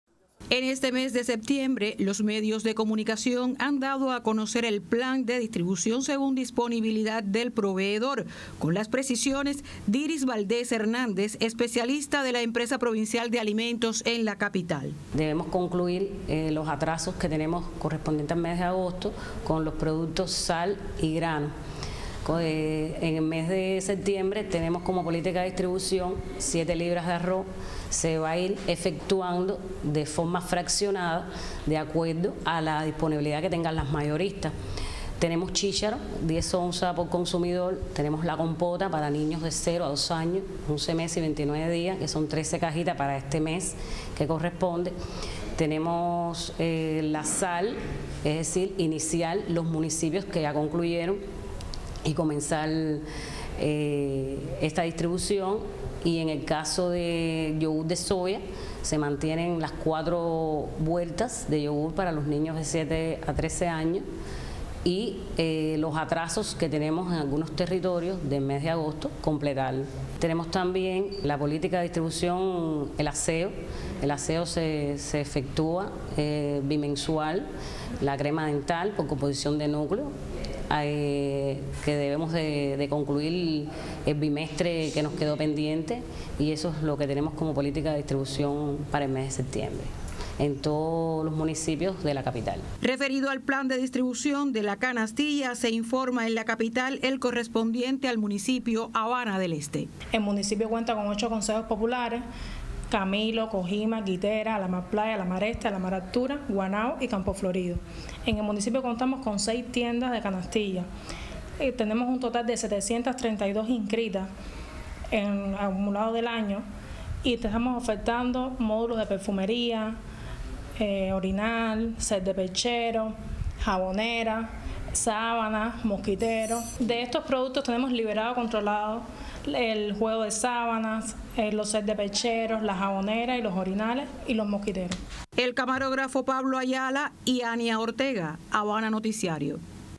Anuncian programa de distribución de los productos de la canasta básica del mes de septiembre.
Desde HabanaNoticiario nos llega el reporte de la periodista Ania Ortega, sobre el programadedistribución de los productos de la canastabásica correspondiente al mes de septiembre, según disponibilidad de los proveedores, el cual ya ha sido informado por los medio de comunicación. Más detalles, a continuación.